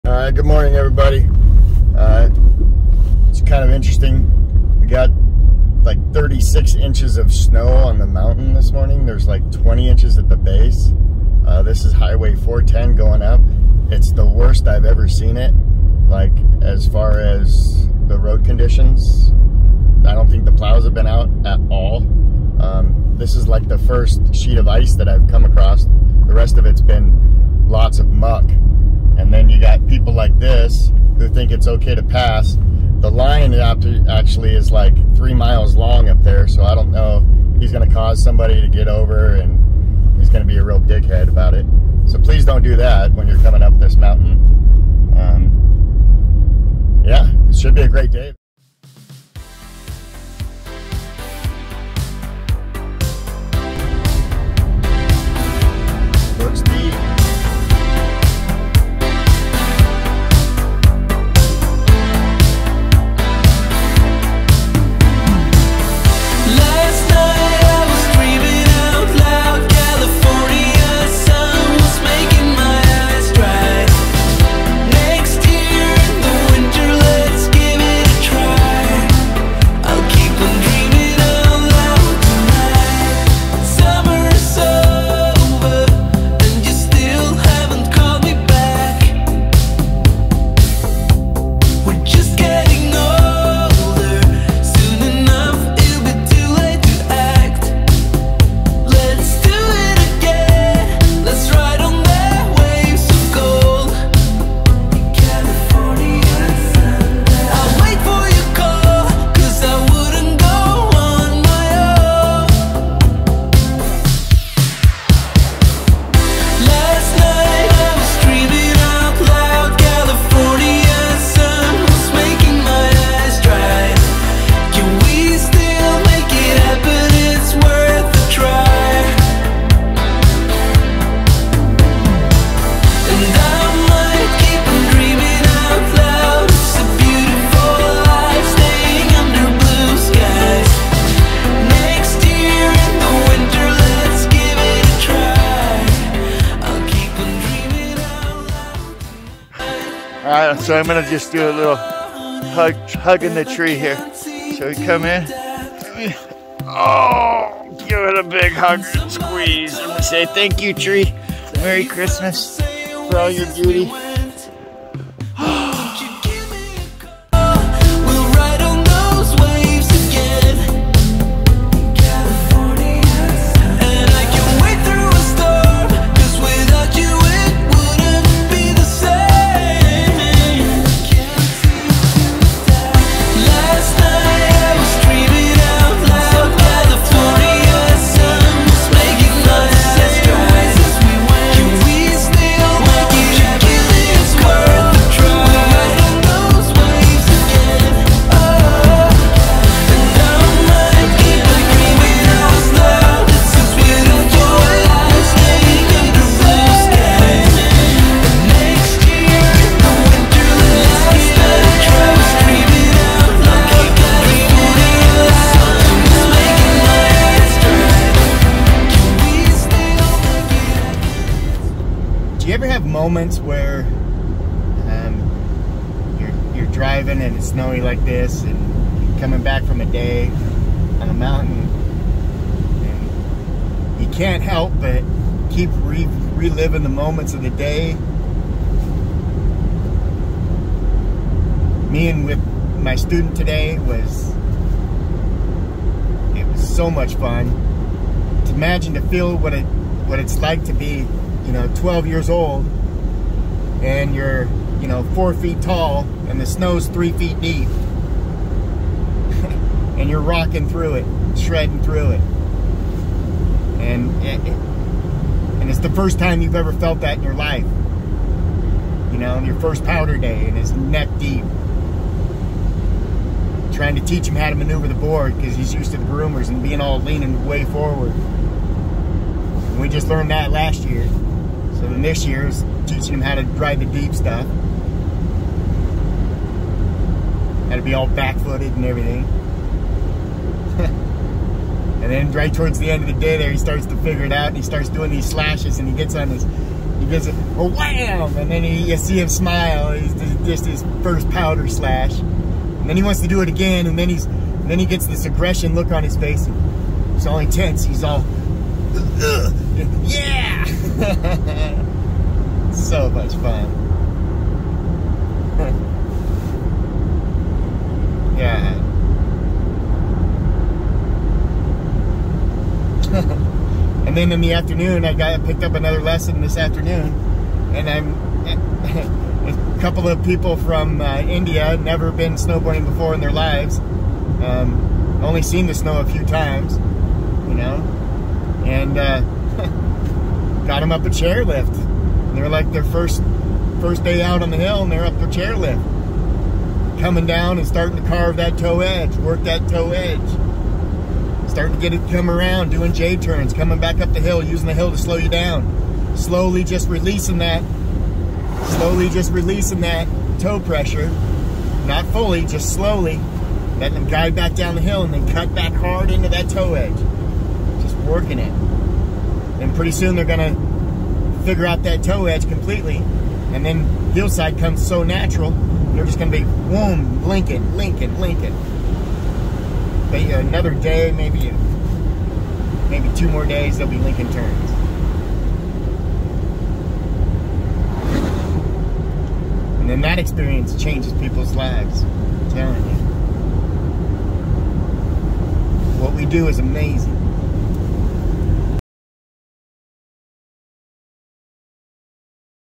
Neversummer powder at crystal Mountain Washington Jan 2 2022
the roads are still there however was the hardest time slow going to get there . the road shut down after i arrived . and stayed closed for 2 hrs of powder riding before the crowds.  did show up .gray day .